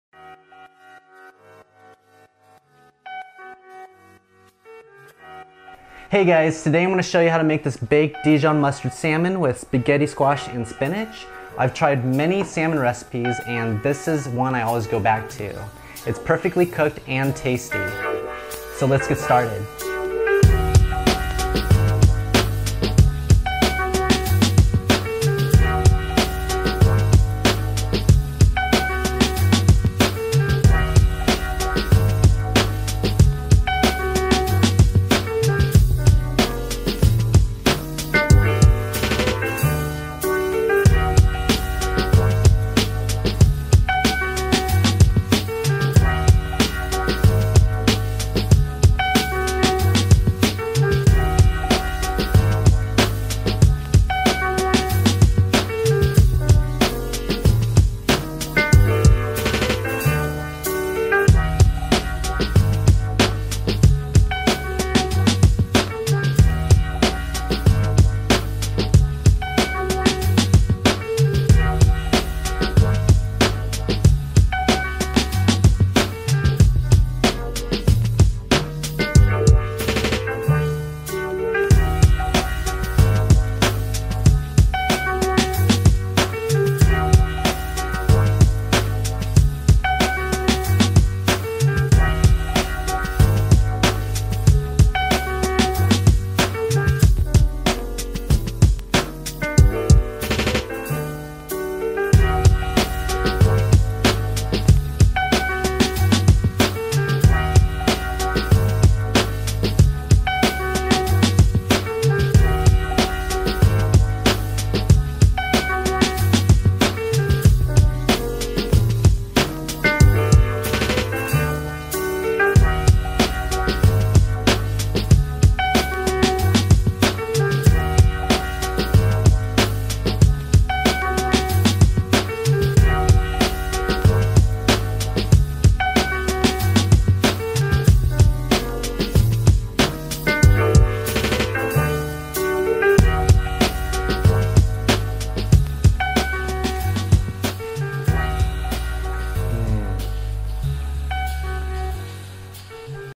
Buttery Soft Baked Dijon Mustard Salmon with 15-Minute Spaghetti Squash and Spinach | TOM TO TABLE
You won't believe how buttery soft this baked salmon with a dijon mustard yogurt sauce is. The sauce really brings out the flavor of the fish and makes it even more indulgent. This is a fool-proof method to make salmon like they do at a fancy restaurant. The salmon really lends itself well to the spaghetti squash and spinach.

Buttery Soft Baked Dijon Mustard Salmon with 15-Minute Spaghetti Squash and Spinach

INGREDIENTS:
- Makes 4 servings
For the salmon:
1 3/4 lb wild salmon, cut into 4 pieces
3 Tbs. extra virgin olive oil
2 Tbs. plain greek yogurt
2 Tbs. whole grain dijon mustard
2 Tbs. fresh lemon juice

For the sides:
2 Tbs. light olive oil
1 small spaghetti squash
2 cups spinach, chopped

Directions:
1. Preheat oven to 375° F.
2. Poke holes on the surface of the spaghetti squash. Place it in a casserole dish and put it in a microwave for 5 minutes to soften the skin.
3. Cut the ends of the spaghetti squash and then in half long ways. If it's too hot use an oven mitt or towel. Remove seeds. Pour water up to 1-inch into the casserole dish. Put one half, cut-side down, into the dish. Microwave for 5 minutes. Repeat with the second half.
4. On a foil-lined baking sheet, drizzle some olive oil and place the salmon skin-side down.
5. Mix the yogurt, dijon mustard, and lemon juice together. Spread evenly on each piece of salmon. Bake for 25 minutes.
6. When the spaghetti squash is done being microwaved, put the halves into a bowl and let it cool down to the touch. Take a fork and scrape the strands out.
7. In a large saute pan on medium heat, pour in light olive oil. When oil shimmers, put the spinach in and stir until wilted.
8. To serve, make a bed of spaghetti squash, salt and pepper, and spinach. Take a spatula and carefully lift the salmon off the skin and top off the dish.

All filmed on a Sony RX100 M4 and Fuji X100T